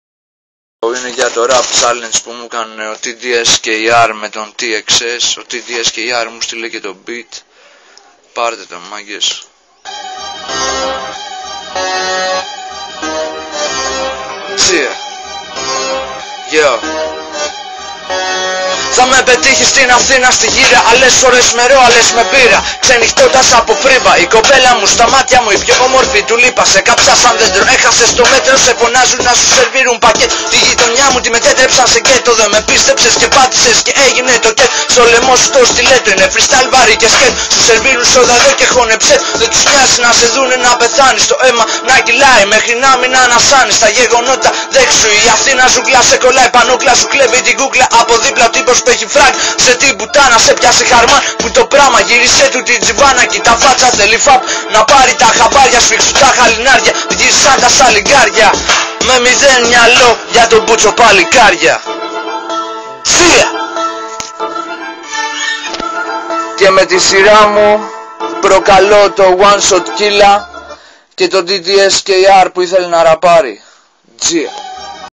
Ramsi - Verse @ Rap Challenge
Αποδέχεται τη πρόκληση του Txs.